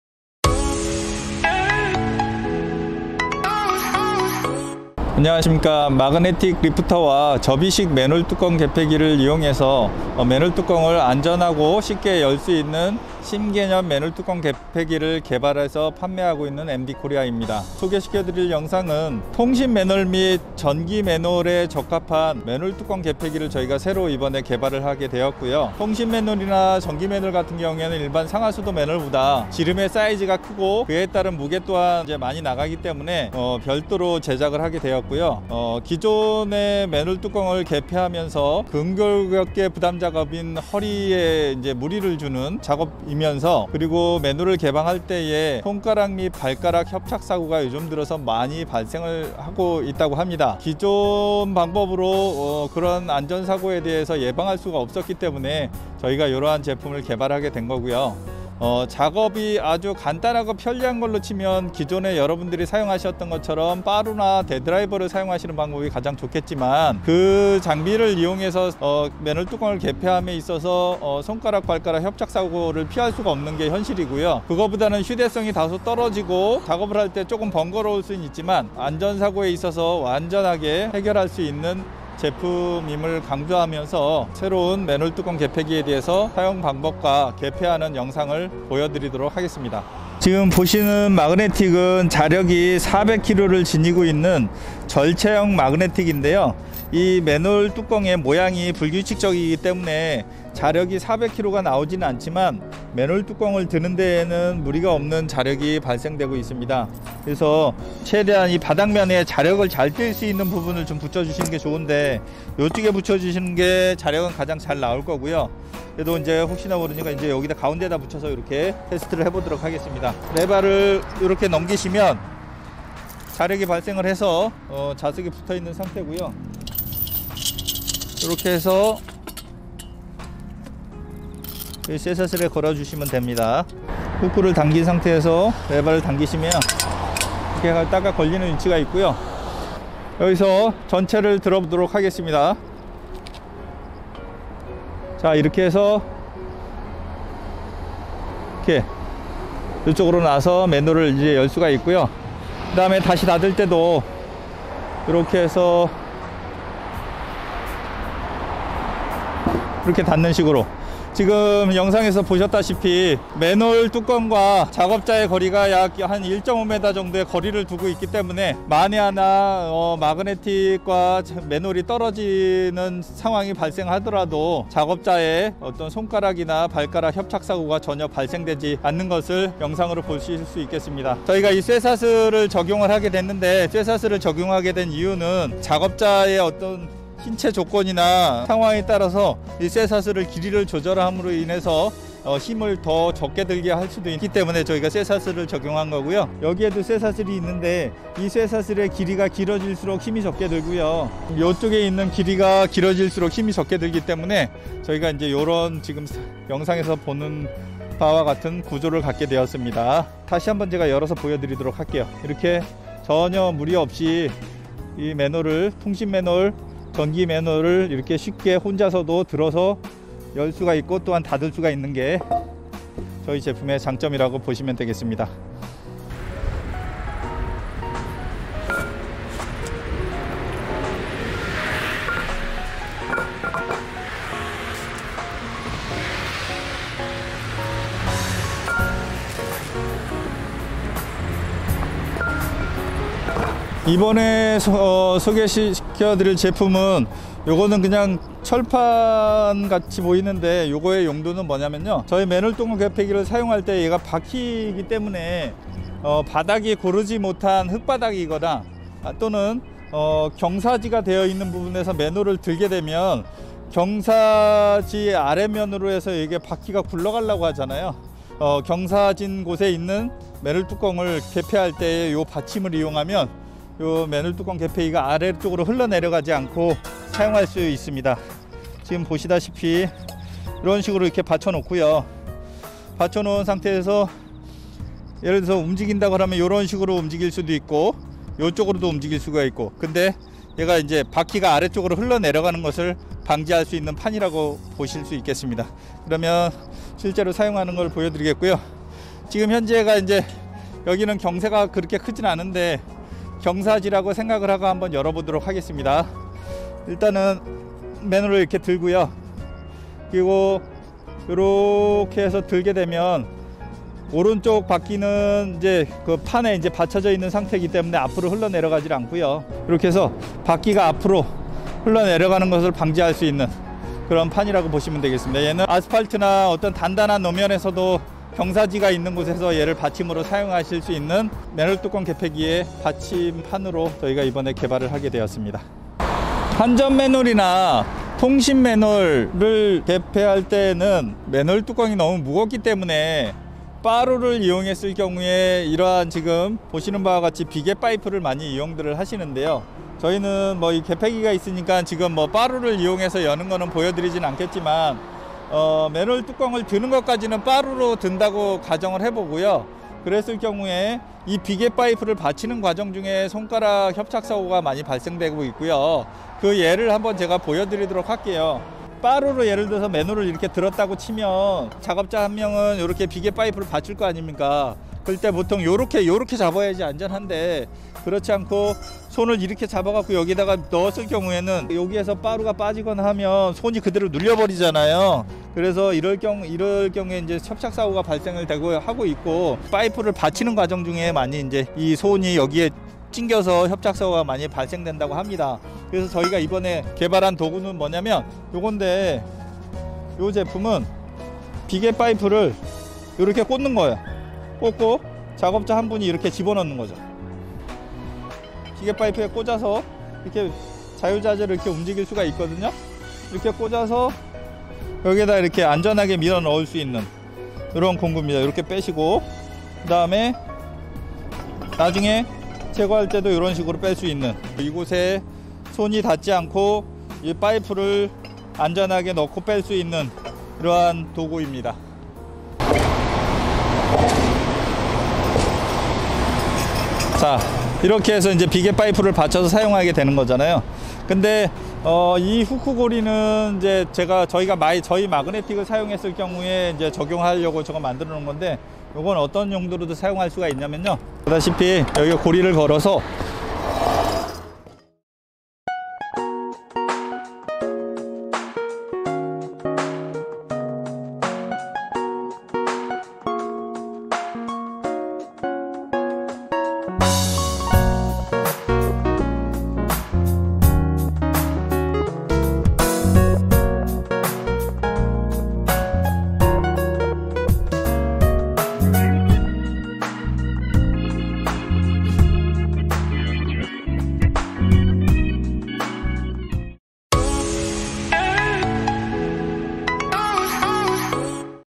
전기통신 맨홀 전용 맨홀뚜껑 개폐기 및 신제품 소개(쉽고 안전하고 빠르게 맨홀뚜껑열기)
안녕하십니까? 국내 최초 ‘맨홀뚜껑 개폐기’ 전문기업 ‘㈜엠디코리아’입니다.

[기존 공·기구의 문제점]
* 근골격계 부담 작업으로써 반복되는 개폐 작업으로 인해 작업자의 허리부상이 발생.
* 대표적으로는 빠루(쇠지렛대)를 이용한 방법으로, 맨홀을 들어올리는 도중, 빠루가 미끄러져
작업자의 손가락 및 발가락 협착사고 발생.

이러한 문제점을 완벽하게 해결한 신개념 ‘맨홀뚜껑 개폐기’를 소개합니다.
빠루가 휴대성이 좋고 간편하다고 하여 기존 방법을 고집하지만, 이러한 간편함이 작업자의 안전보다 우선시 될 수는 없습니다.

‘㈜엠디코리아’가 개발한 신개념 ‘맨홀뚜껑 개폐기’는 약 2년에 걸쳐, 한국수자원공사 성과공유제 과제를 수행하면서 작업자의 안전을 지켜줄 수 있는 제품으로 인정받아 계약을 체결하였습니다.

엄격하게 검증받은 제품으로 믿고 구매하셔도 되며, 특허받은 제품으로서 ‘수의계약’으로 구매가 가능합니다.

** 맨홀 뚜껑이 찌들어서 열지 못하여 맨홀 공사를 다시 해야 할 맨홀 뚜껑을 공사 없이 열 수 있는 특허기술을 보유하고 있습니다. 공사비용과 시간을 절약할 수 있습니다.

제품 및 시연 문의는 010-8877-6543